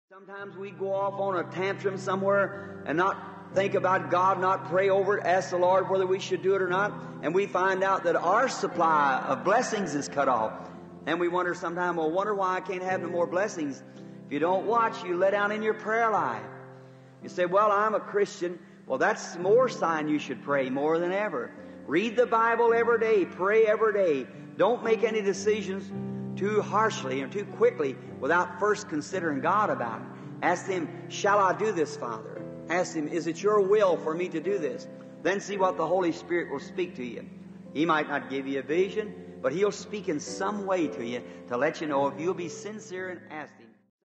"The Prophet Elisha" Rv. William Branham
54-0723

The Prophet Elisha

Sometimes we go off on a tantrum somewhere and not think about God, not pray over it, ask the Lord whether we should do it or not, and we find out that our supply of blessings is cut off. Then we wonder sometime, “Well, wonder why I can't have no more blessings.” You don't watch, you let down in your prayer life. You say, “Well, I'm a Christian.” Well, that's the more sign you should pray, more than ever. Read the Bible every day. Pray every day. Don't make any decisions too harshly or too quickly, without first considering God about it. Ask Him, “Shall I do this, Father?” Ask Him, “Is it Your will for me to do this?” Then see what the Holy Spirit will speak to you. He might not give you a vision, but He will speak in some way to you, to let you know, if you'll be sincere and ask Him.

📖 Daily Bread

Jeremiah 29:13

And ye shall seek me, and find me, when ye shall search for me with all your heart.